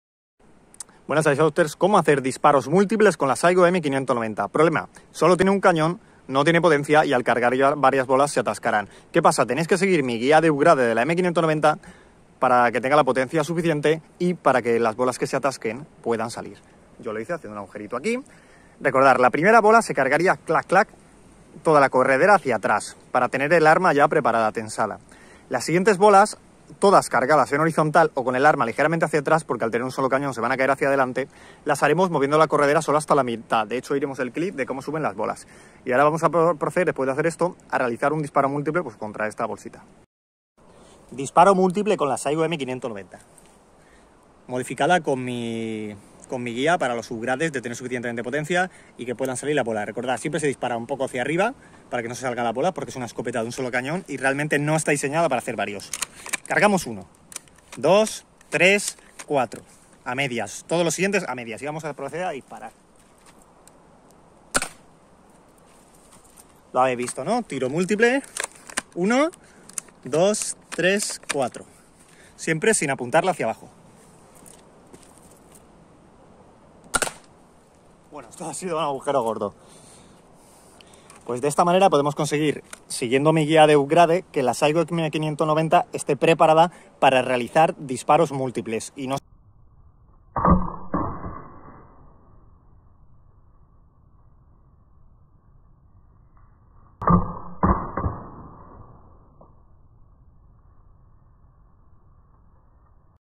⚪(AGM)SAIGO M590 ¡COMO HACER DISPARO MULTIPLE! upgrade necesario*
***Modifiicar la escopeta lo hace cada uno bajo su propio riesgo.
***Se necesitan los UPGRADES de este video para poder usar el DISPARO MULTIPLE sin que se rompa y con potencia:
*** PRIMERO, REALIZAR EL AUGUJERO JUNTO AL GATILLO PARA QUE LAS BOLAS QUE SE ATASQUEN AL CARGAR VARIAS PUEDAN SALIR.

Utilizar siempre gafas de protección.